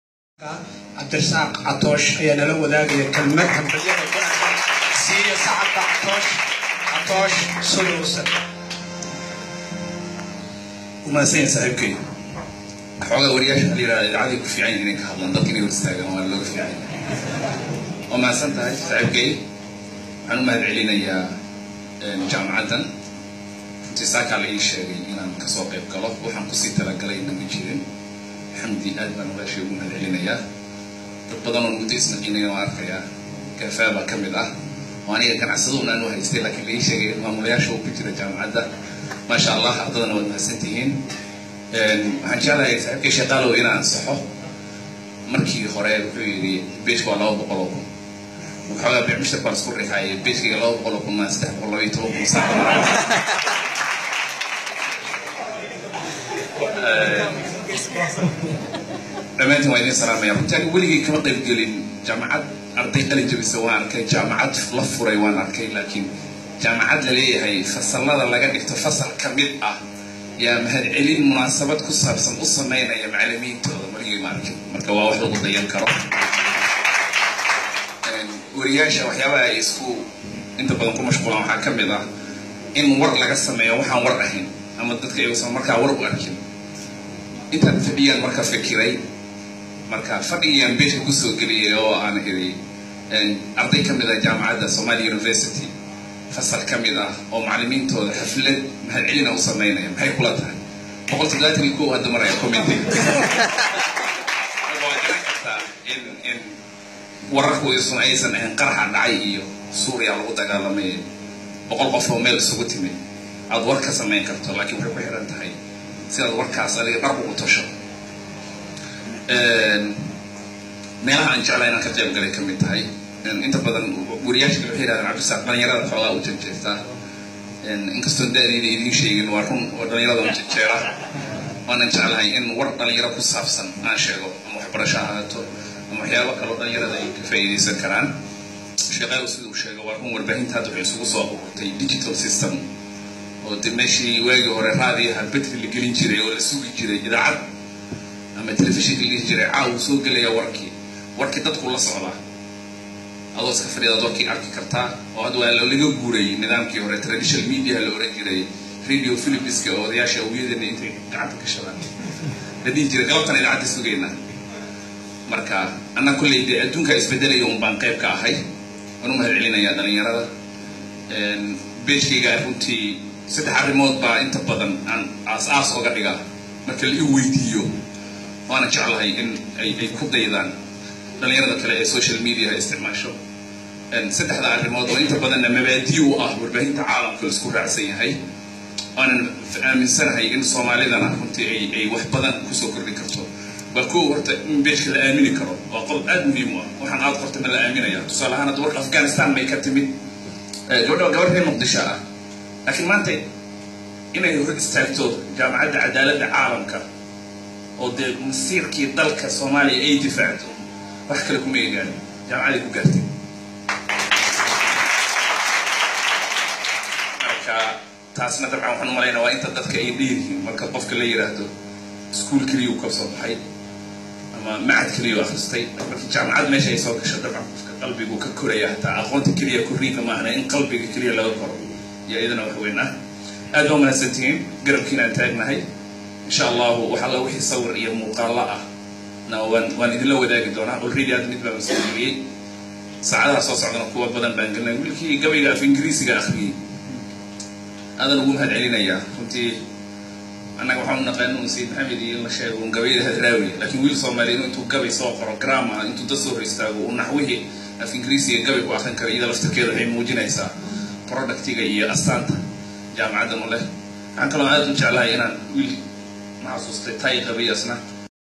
Cabdirisaaq Xaaji Catoosh oo lagu casumay xaflad ardayada jamacada somaliya uqabteen macalimiintoda
Cabdirisaaq Xaaji Catoosh oo lagu casuumay xaflad ardayada jamacada somalia uqabteen macalimiintooda